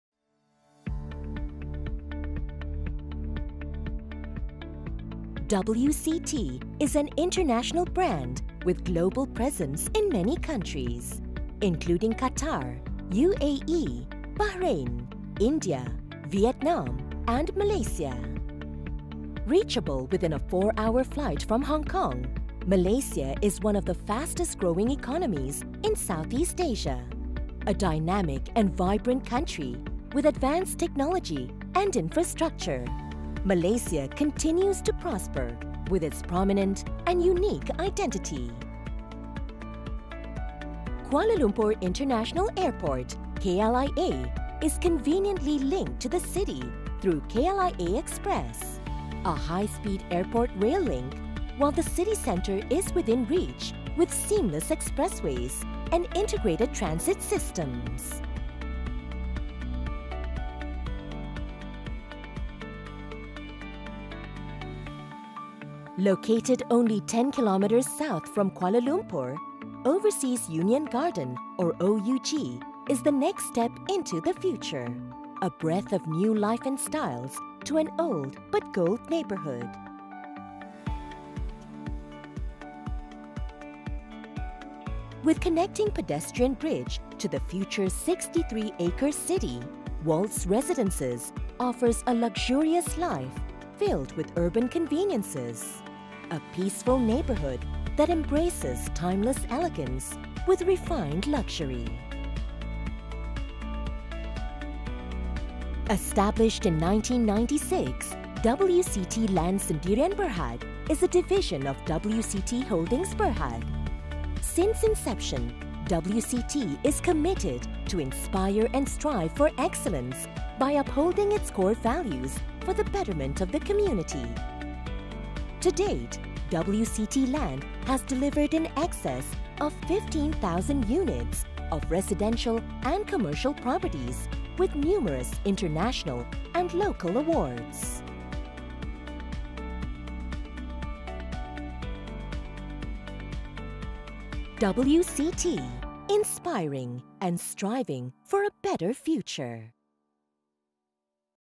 Experience an all-embracing elegance at Waltz Residences
Experience an all-embracing elegance at Waltz Residences, a freehold iconic lifestyle where 419 units of effortlessly refined luxury are crafted for the flourishing urban community. With 38 floors on the North Tower and 33 floors on the South Tower, Waltz Residences merges urban chic architecture and conveniences with an upscale retail mall just a few steps away from home and the rest of Klang Valley just within half an hour’s reach.